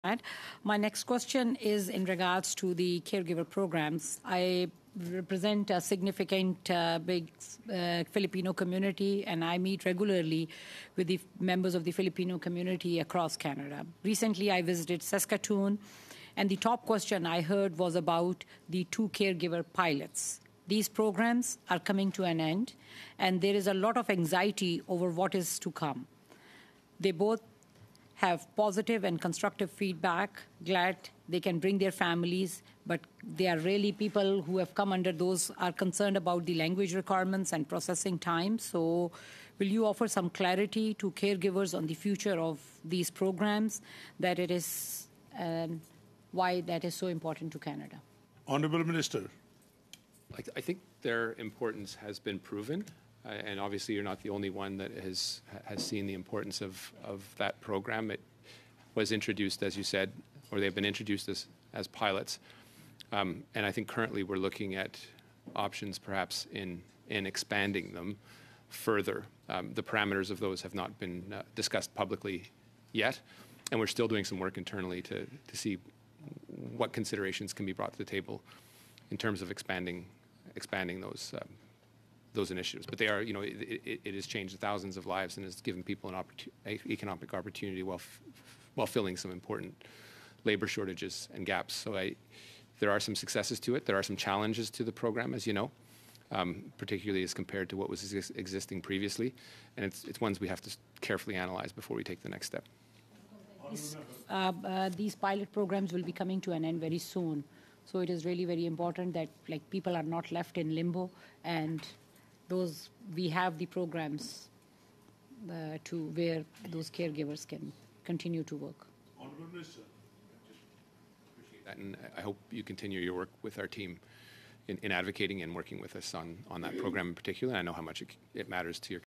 CIMM: Caregiver pilot programs
I asked Immigration Minister Marc Miller if he can provide some clarity to people concerned about the expiry of the two caregiver pilot programs and what is coming next.